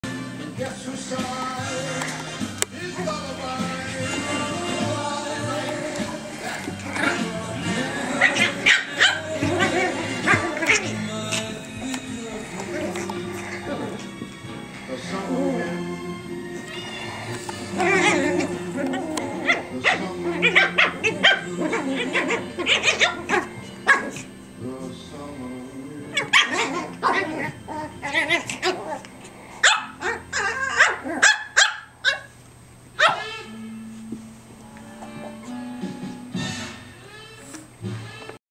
The Biewer Sisters - Rumble under the StepStool
Isabelle & Imaree rumbling under the step stool as Corky watches from the sofa.  Video taken in February 2007